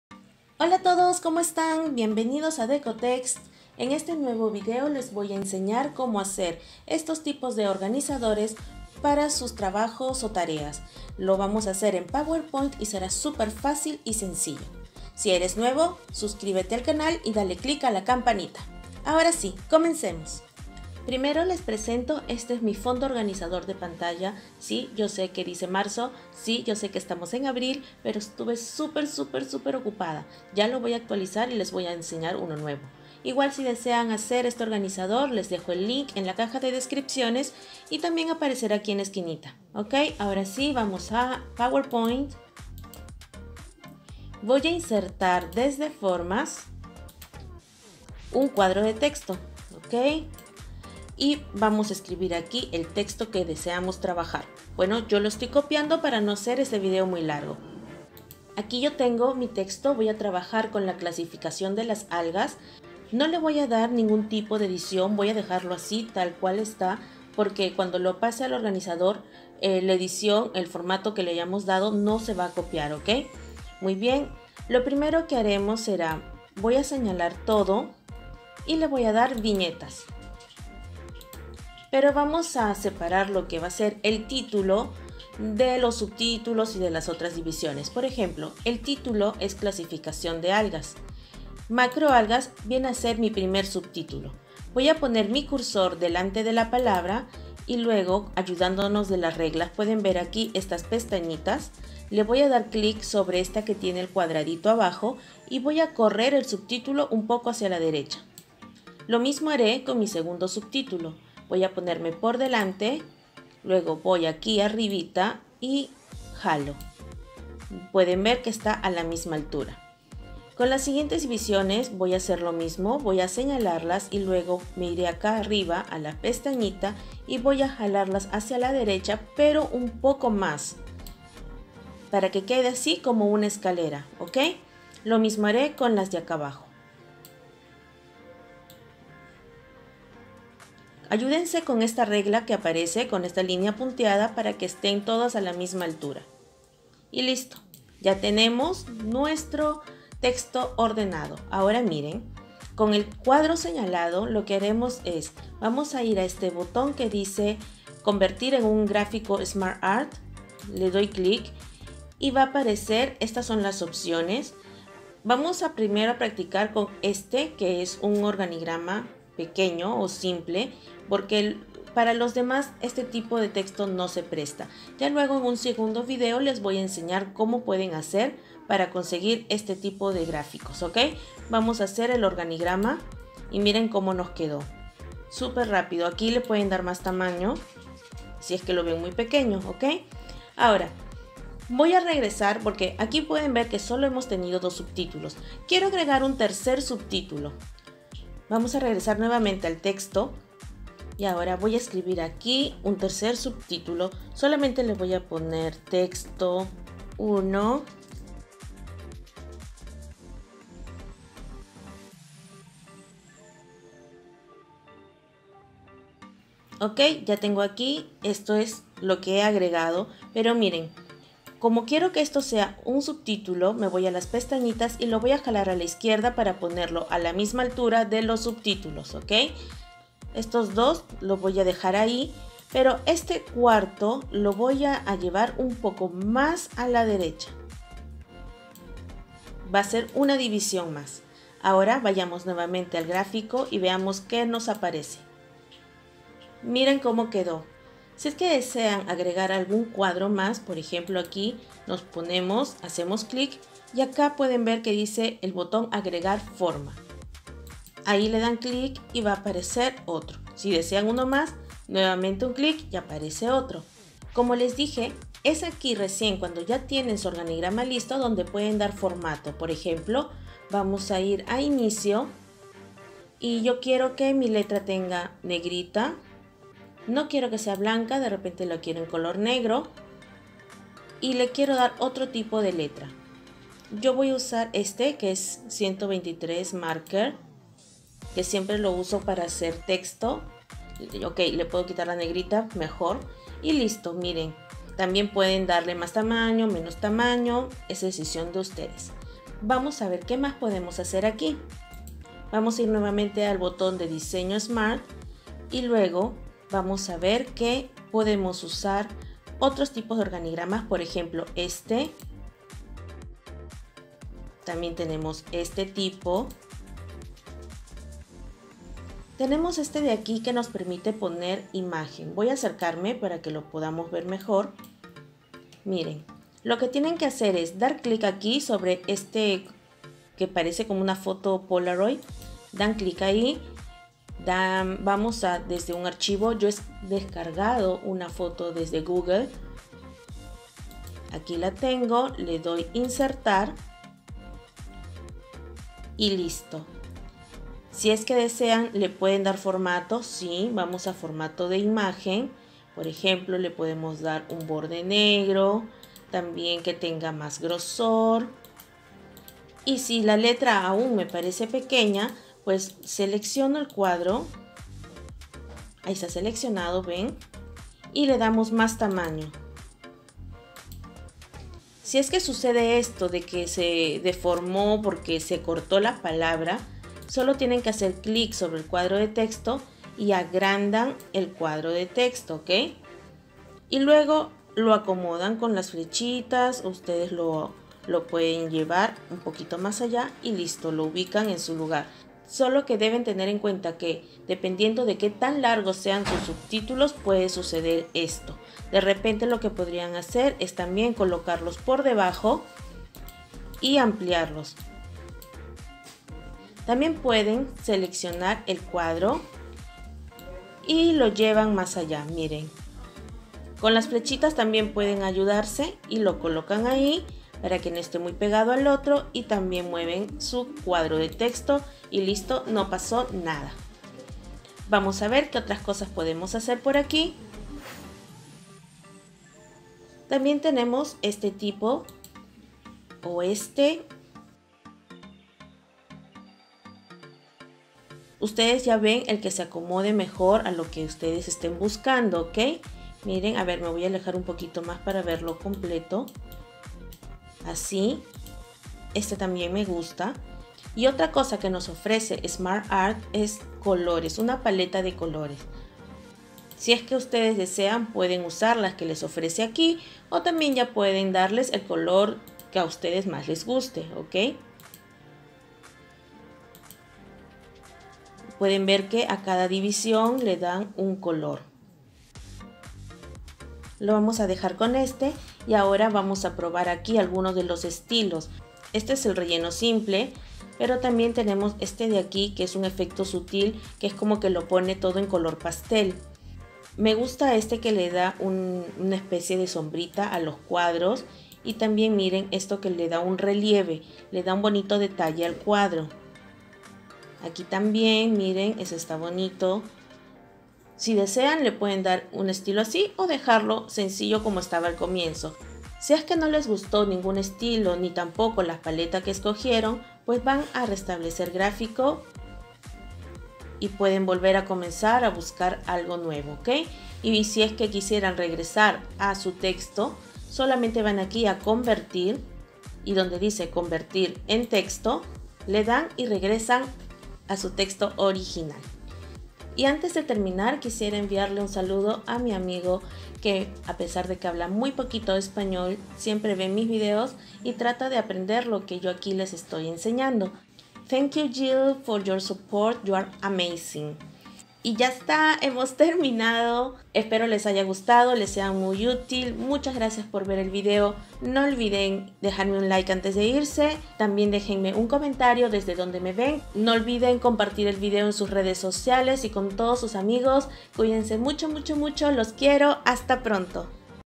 COMO HACER UN MAPA CONCEPTUAL/ORGANIZADOR /ORGANIGRAMA EN POWERPOINT FÁCIL Y RÁPIDO CON SMARTART
Hola a todos, quiero enseñarles como pueden hacer este mapa conceptual  de una manera rápida y sencilla, así ahorraras tiempo para hacer tus tareas.

mapas hermosos
mapas faciles
mapas conceptuales fáciles
how to